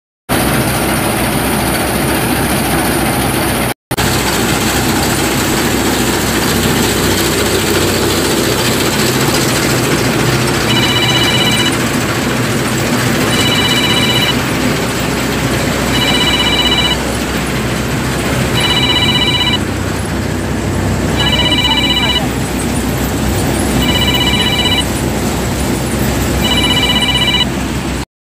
mesin combi maxxi bimo 102L ganti baket untuk jagung!
mesin combi maxxi bimo 102L di ganti baket ya untuk panen jagung